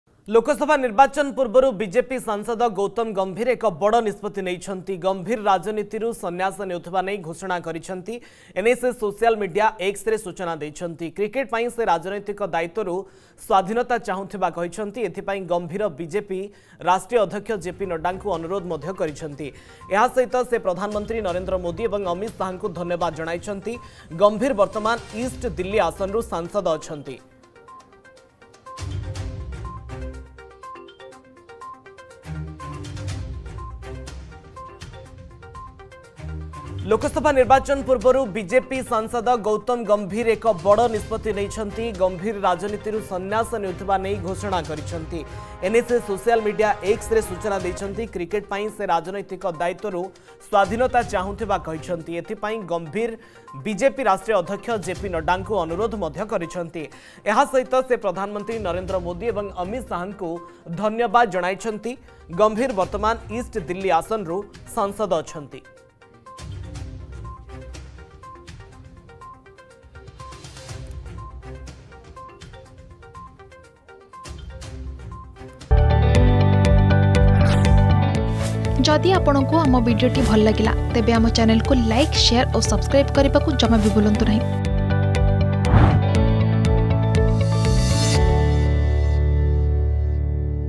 ରାଜନୀତିରୁ ଗୌତମ ଗମ୍ଭୀରଙ୍କ ସନ୍ୟାସ ! || Gautam Gambhir
ଲୋକସଭା ନିର୍ବାଚନ ପୂର୍ବରୁ ବିଜେପି ସାଂସଦ ଗୌତମ ଗମ୍ଭୀର ଏକ ବଡ ନିଷ୍ପତ୍ତି ନେଇଛନ୍ତି। ଗମ୍ଭୀର ରାଜନୀତିରୁ ସନ୍ୟାସ ନେଉଥିବା ନେଇ ଘୋଷଣା କରିଛନ୍ତି। ଏନେଇ ସେ ସୋସିଆଲ ମିଡିଆ ଏକ୍ସରେ ସୂଚନା ଦେଇଛନ୍ତି।